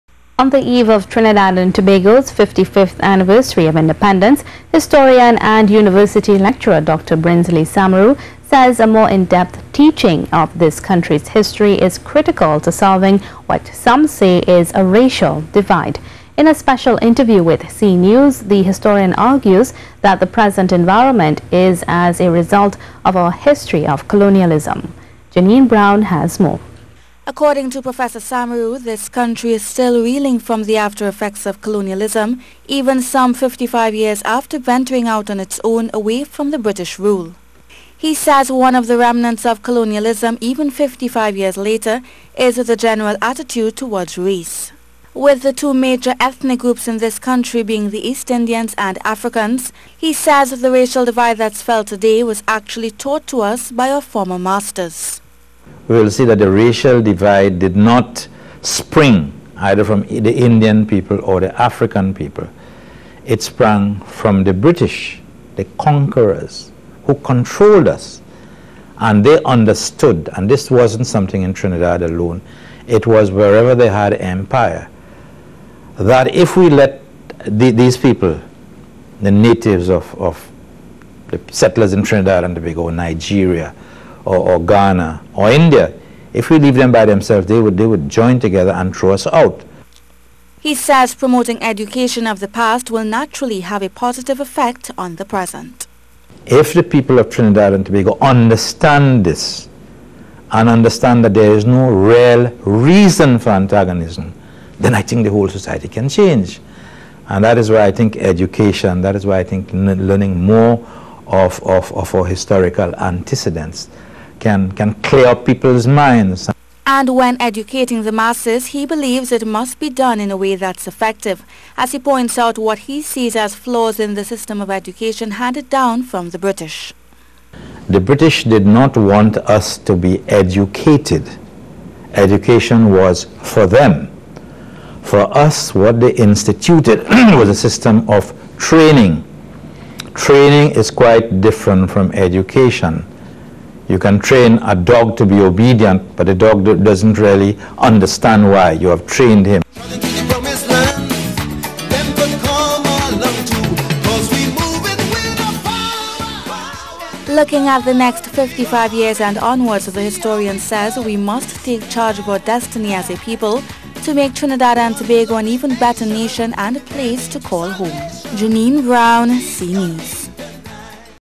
Historian Looks At The After Effects Of Colonialism
On the eve of Trinidad and Tobago's 55th anniversary of Independence, Historian and University Lecturer, Dr. Brinsley Samaroo, says a more in-depth teaching of this country's history is critical to solving what some may say is a racial divide.